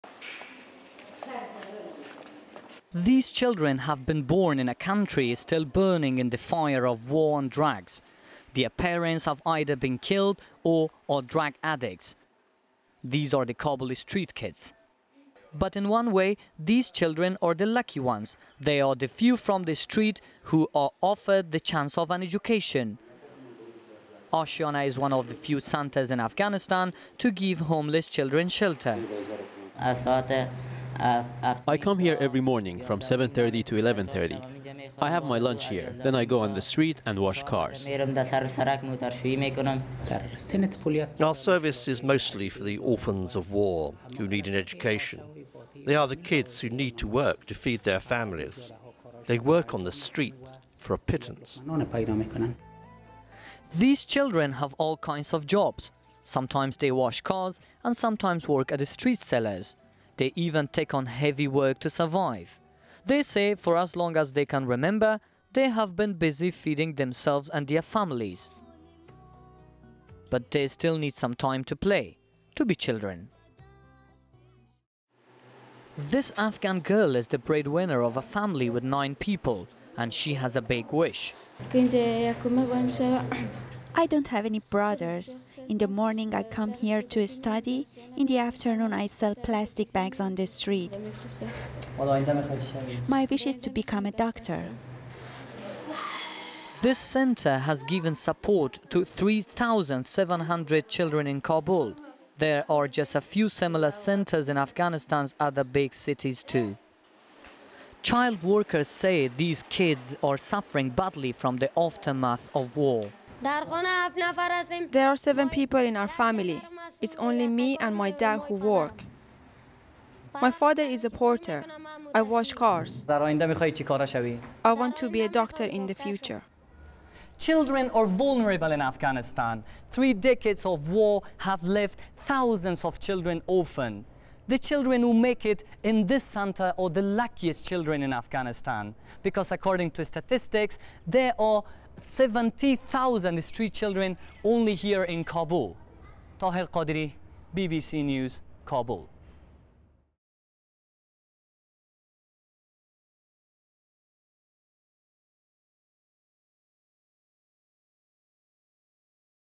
Aschiana, haven to Kabul street children
There are thousands of Afghan orphans in Kabul, who have been badly affected by war. Most of them have lost their parents and have ended up begging on the streets. Tahir Qadiry reports on a school which is giving them an education and has changed their life.